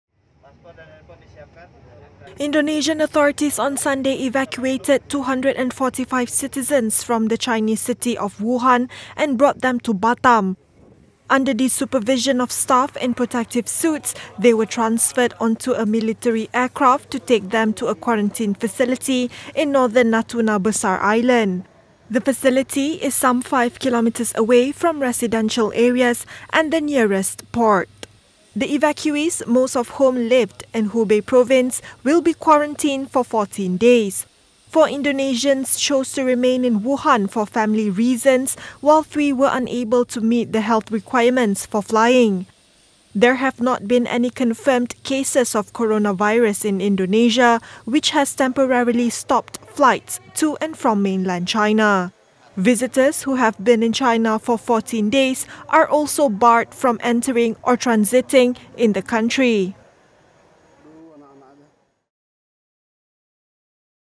Indonesia evacuates 245 citizens from virus-hit Wuhan
Indonesian authorities on Sunday evacuated 245 citizens from the Chinese city of Wuhan and brought them to Batam.

They were transferred to a quarantine facility in northern Natuna Besar island where they will be held for 14 days.

LIKE & FOLLOW US for the latest updates